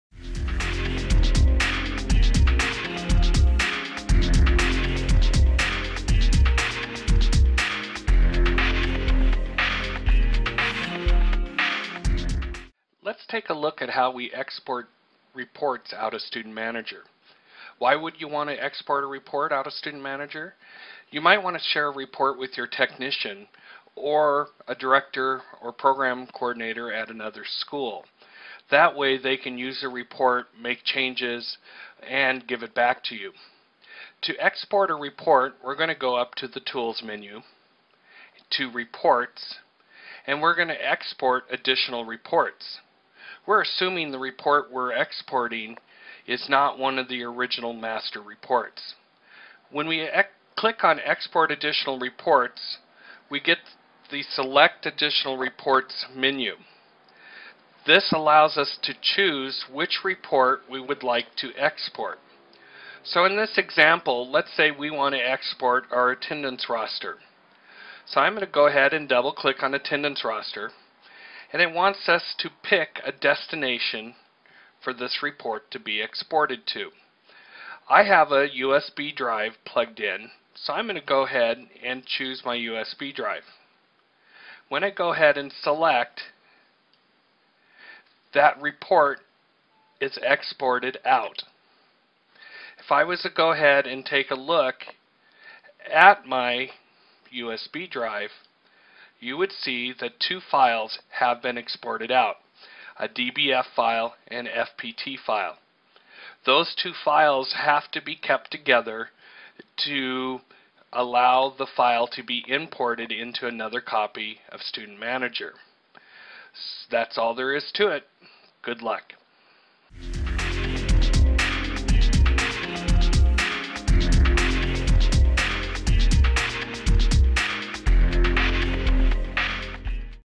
Exporting Reports From Student Manager
A short video explaining how to export reports out of Student Manager.